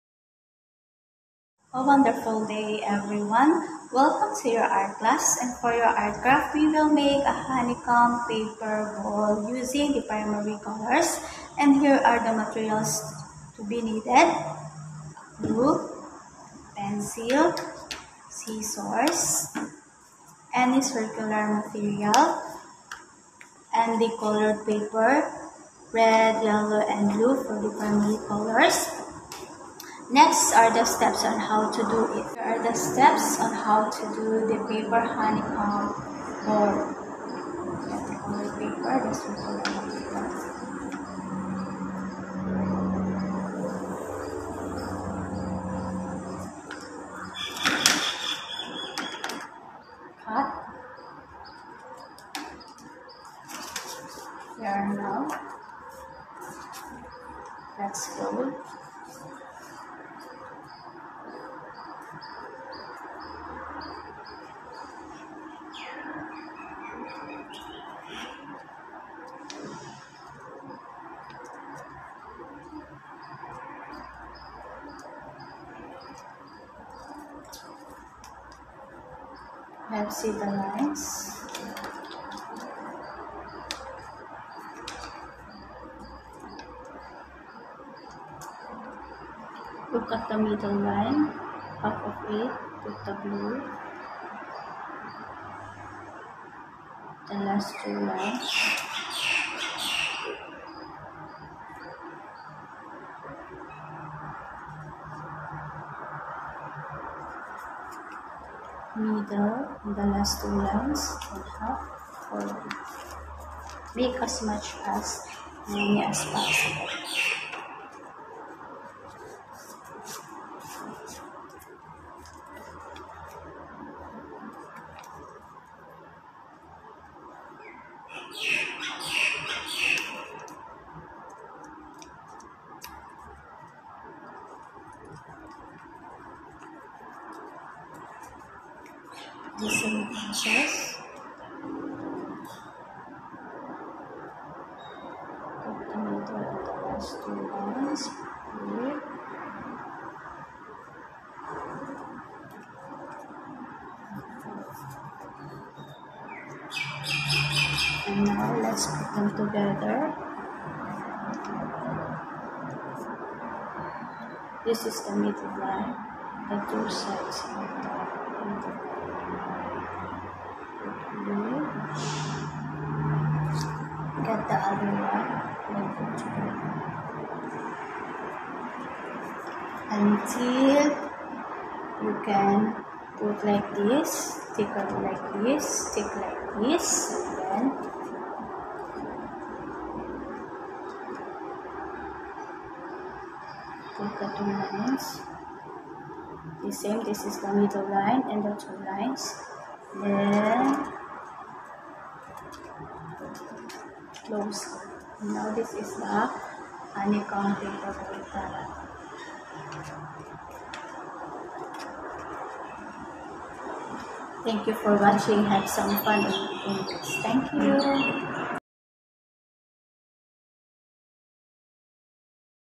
Honeycomb Ball
Art Education Grade 6
Wattanadaroonwit School - Intensive English Programme
Online Learning - June 2021